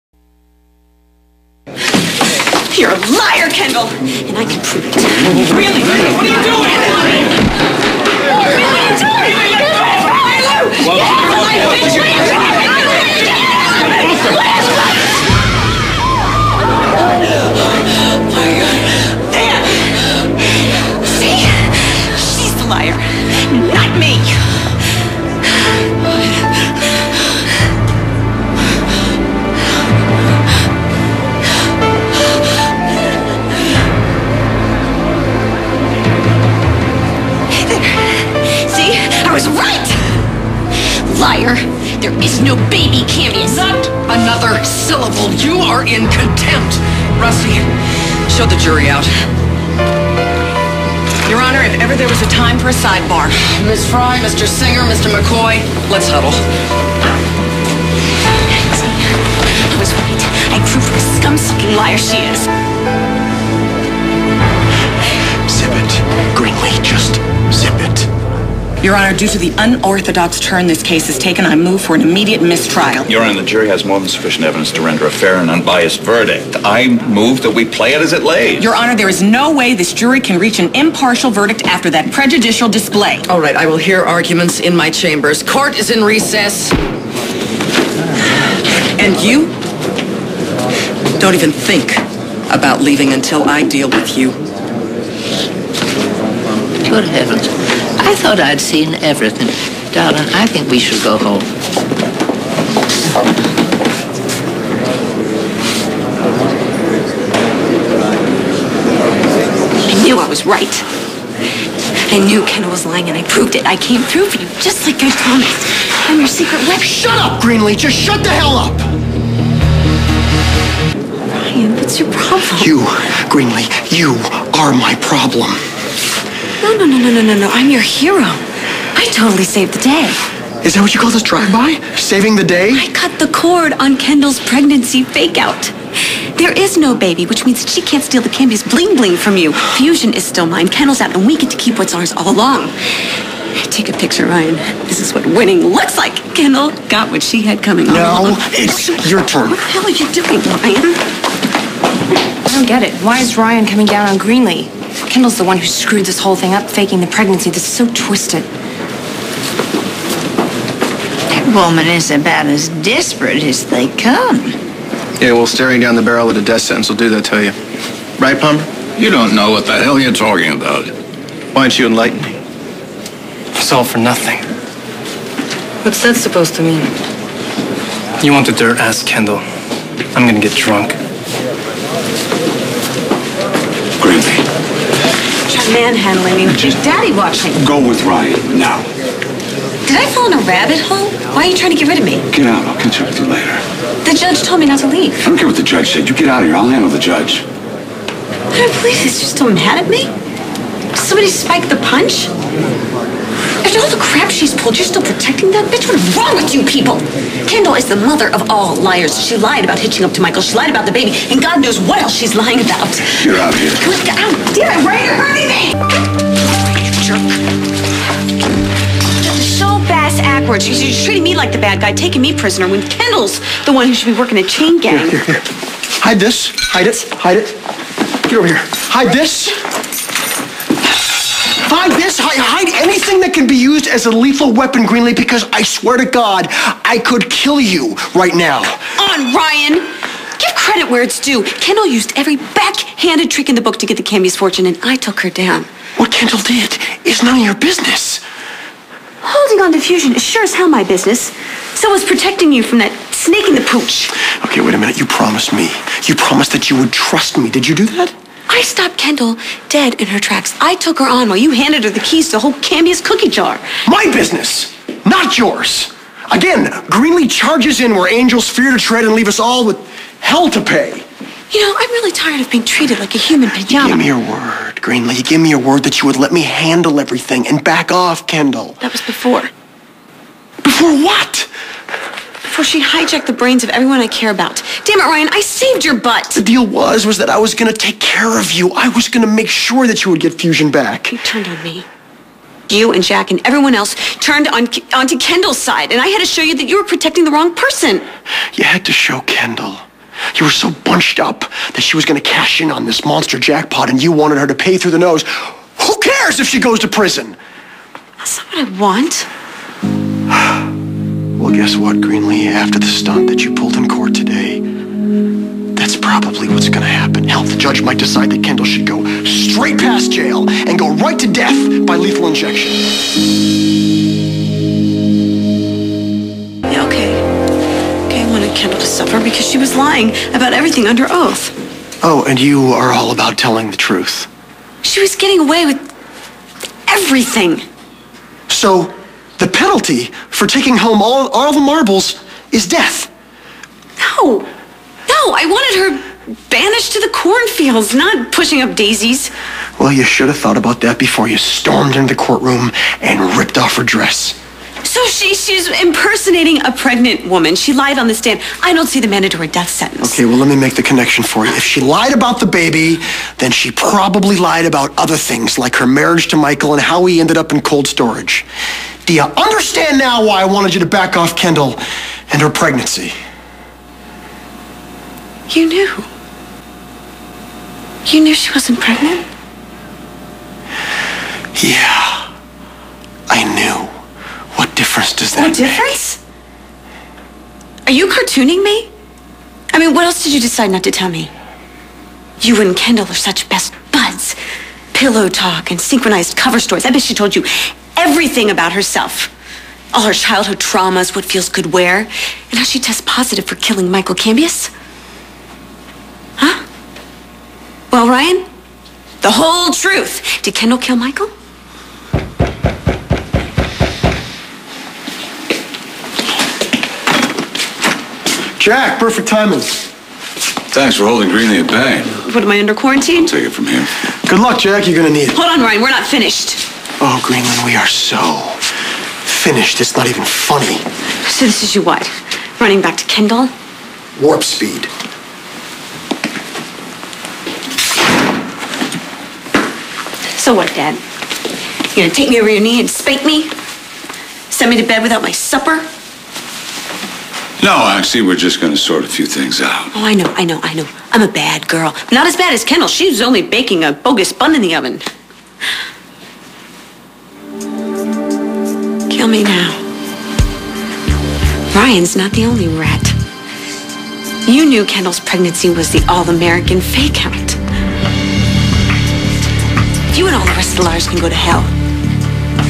Rylee ♥ Kendall Exposed
After Greenlee exposes Kendall's fake pregnancy to the entire courtroom, Ryan takes Greenlee back to her penthouse and expresses his anger at what she did.